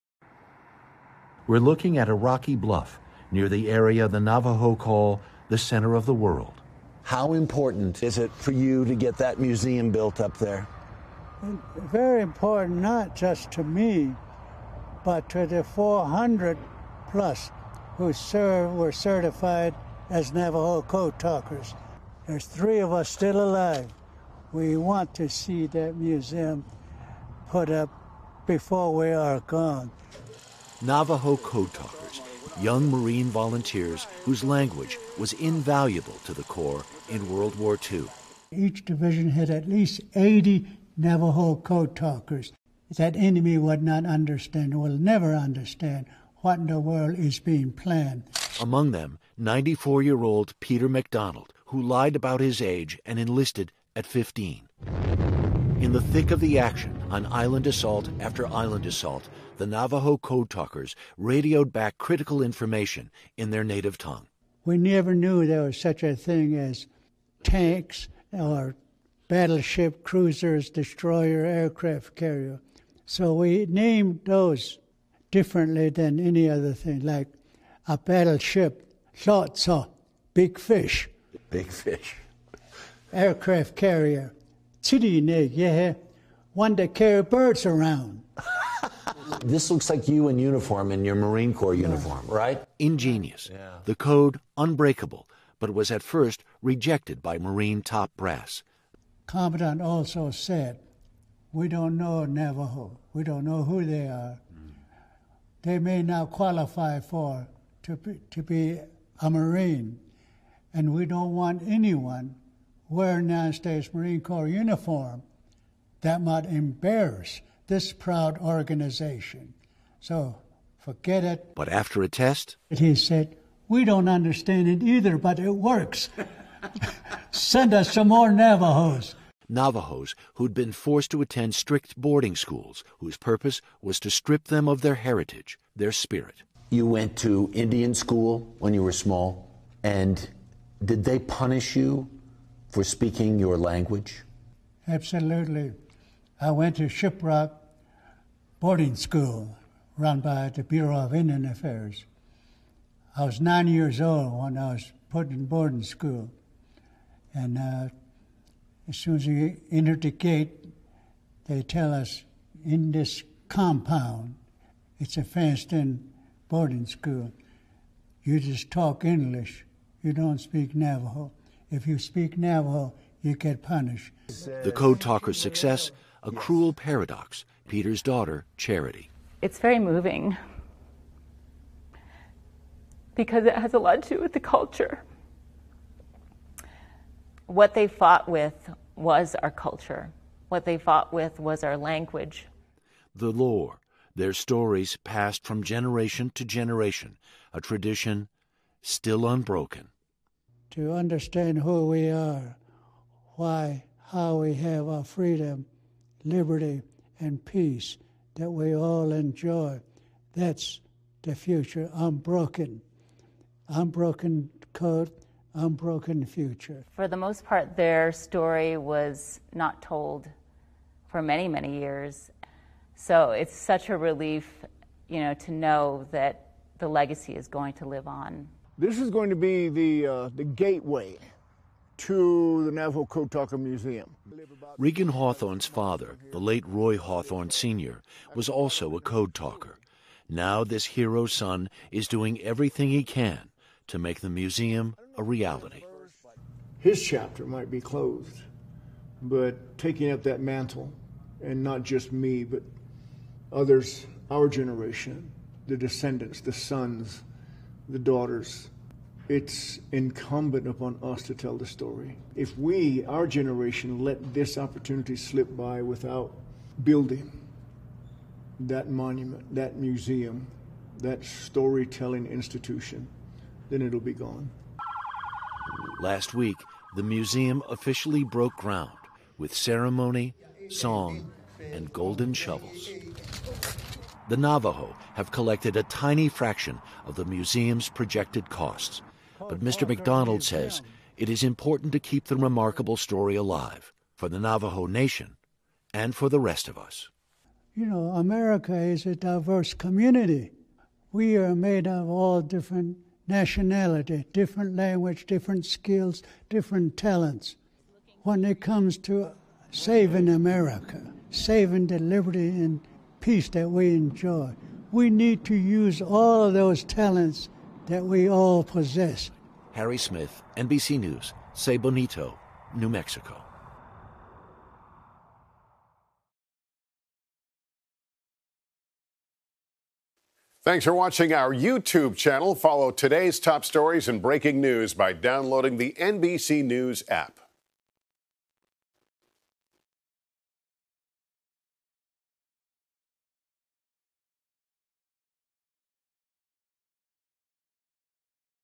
Navajo Code Talkers’ WWII Legacy To Be Immortalized In New Museum
Ground has been broken on a new museum honoring Navajo Code Talkers. At age 94, Peter MacDonald is one of three still alive today. Their service was pivotal in winning World War II, but Marine Corps top brass initially rejected the idea, saying they “might embarrass this proud organization,” MacDonald recalls. Their success was a cruel paradox in light of America’s painful history of Native American boarding schools meant to strip students of their heritage.